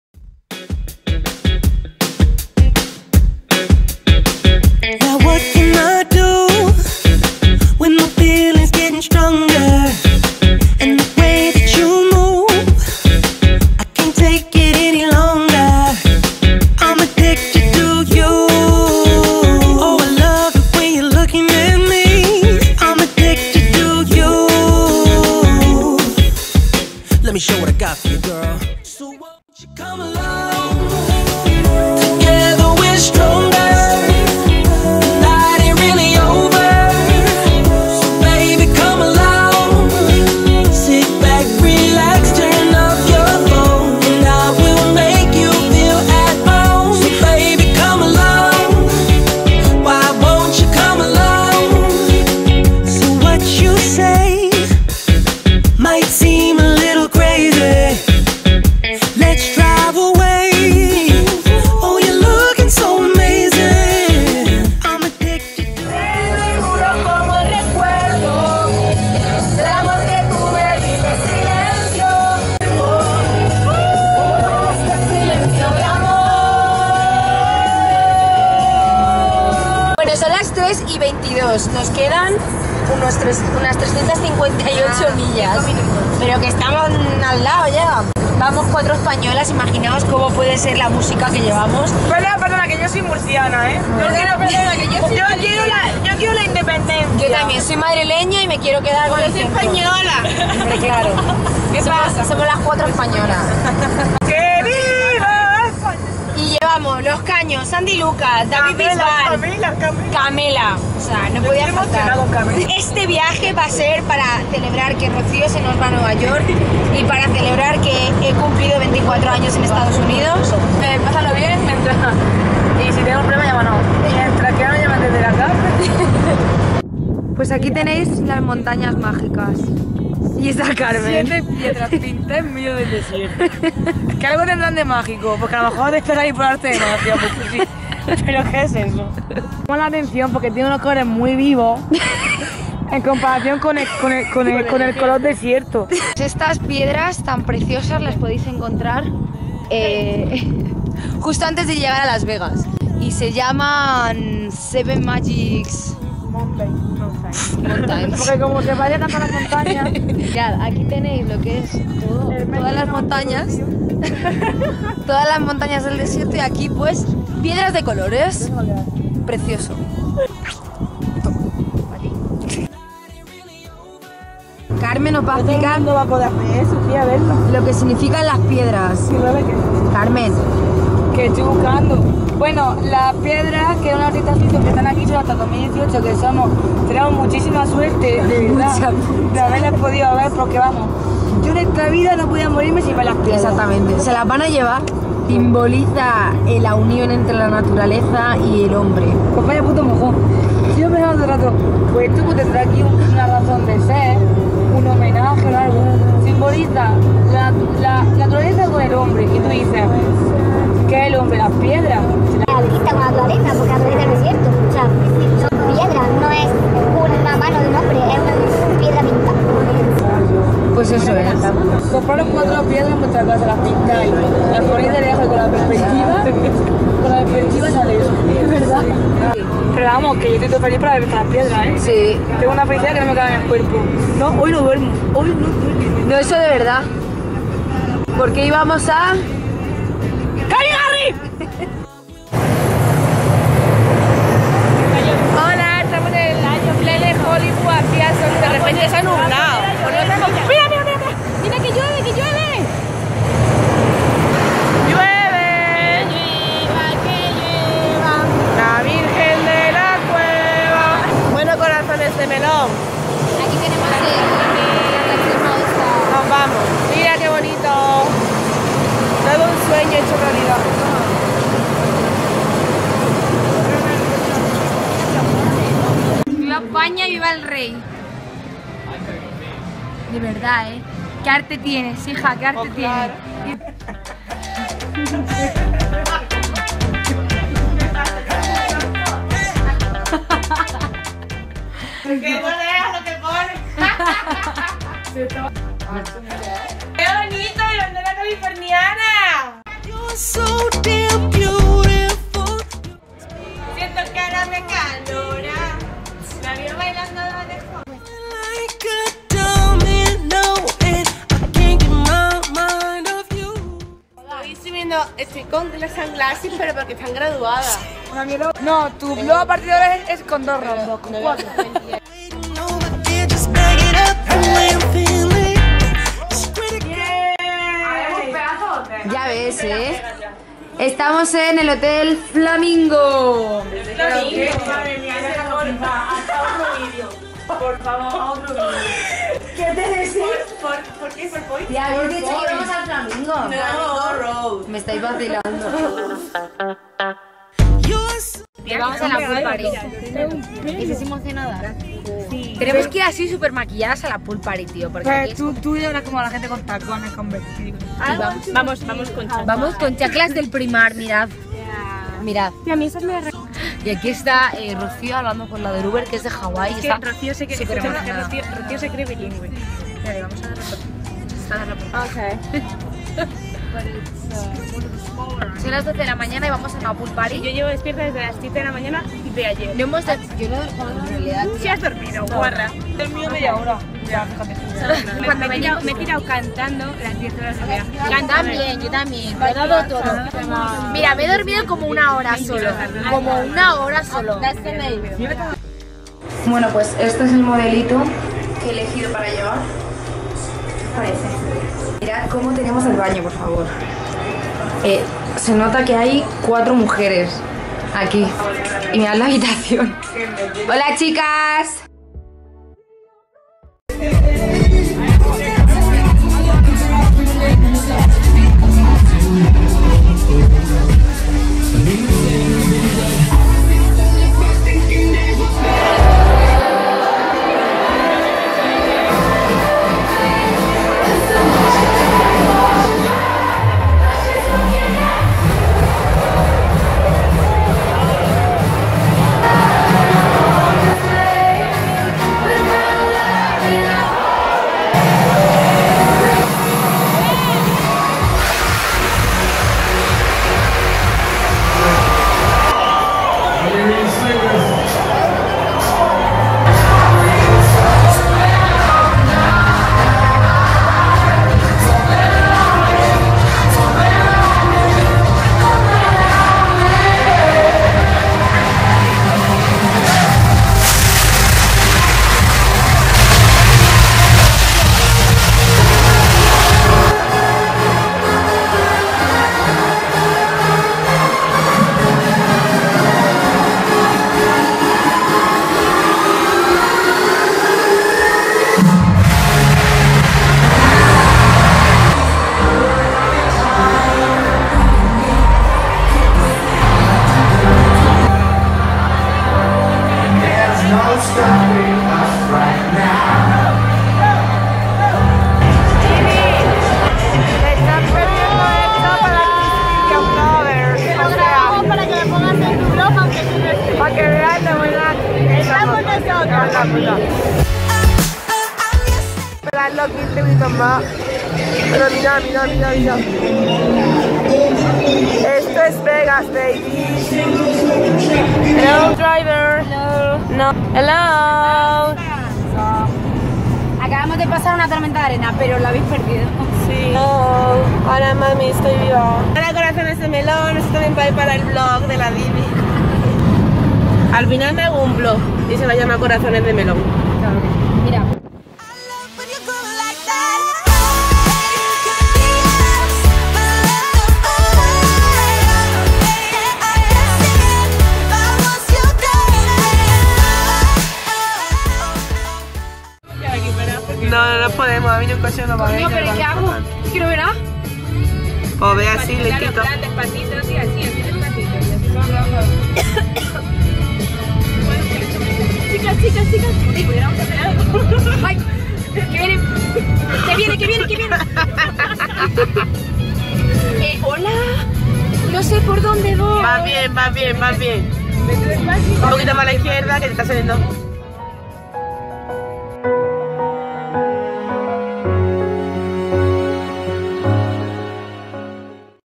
Mi cumpleaños en LAS VEGAS!!|Calvin Harris|DEATH VALLEY
Un cumpleaños que recordaré siempre!!!
En este vídeo podrás encontrar:
-SEVEN MAGIC MOUNTAINS
-CASINOS LAS VEGAS
-CALVIN HARRIS EN OMNIA
-POOL PARTY EN MARQUEÉ
-DEATH VALLEY
-DUNAS DEL DESIERTO
-TORMENTA DE ARENA